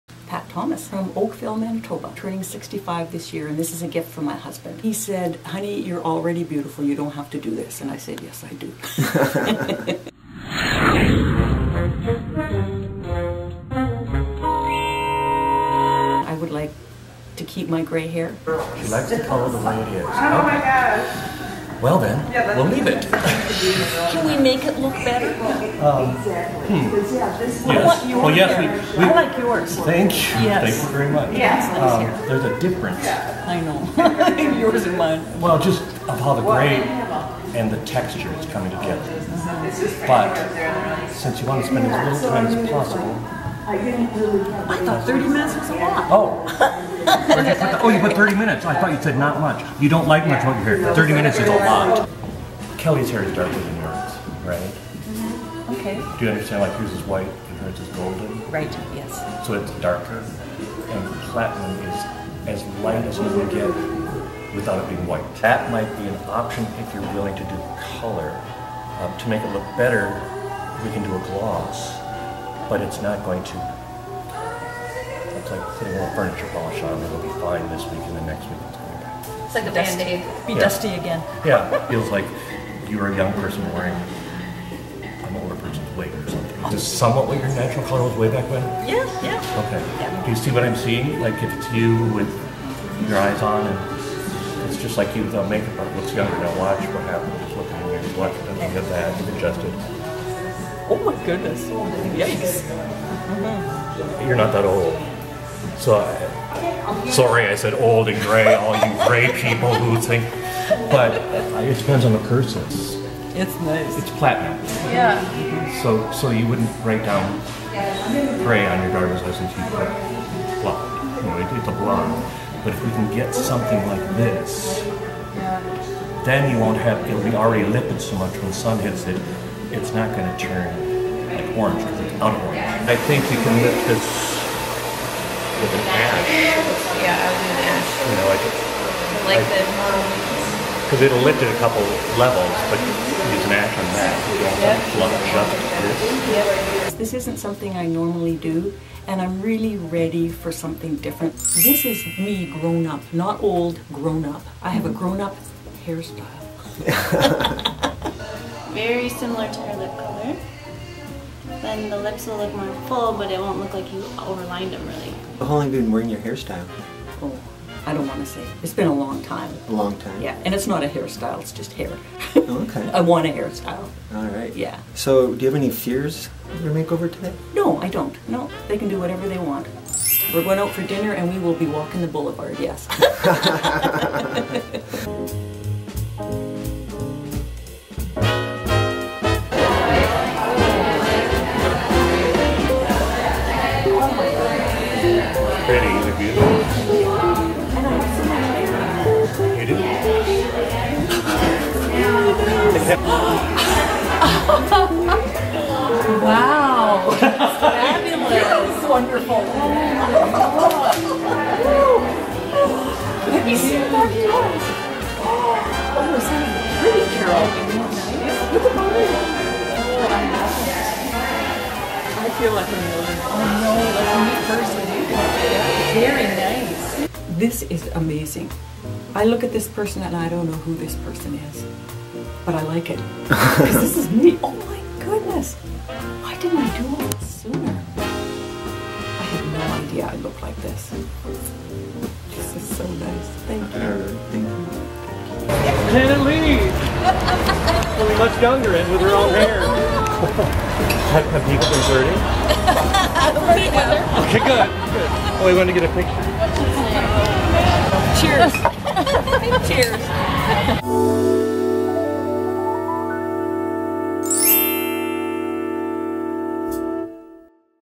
Should She Color Her Gray Hair - MAKEOVERGUY Makeover
Pat was lovely and indecisive about her gray. I felt it just wasn't pretty enough of a gray and that she has enough time later to decide for the less maintenance route.

We need your help! Your support keeps us going.